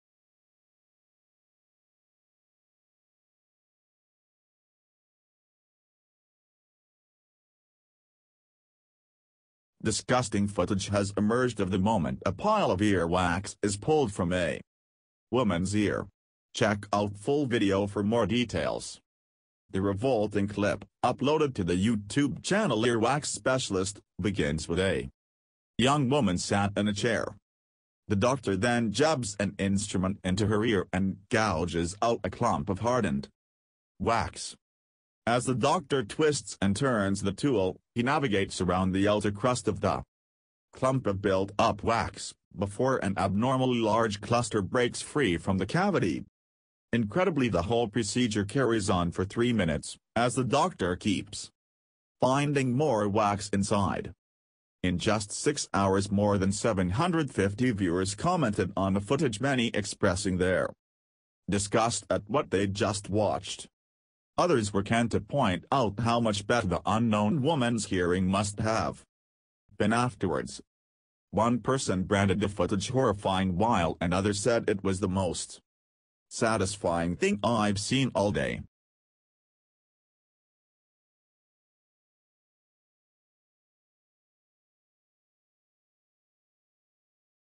Disgusting Moment! Huge Ear Wax Build-Up Is Pulled From Woman's Ear
Disgusting footage has emerged of the moment a pile of ear wax is pulled from a woman's ear . Check out full video for more details.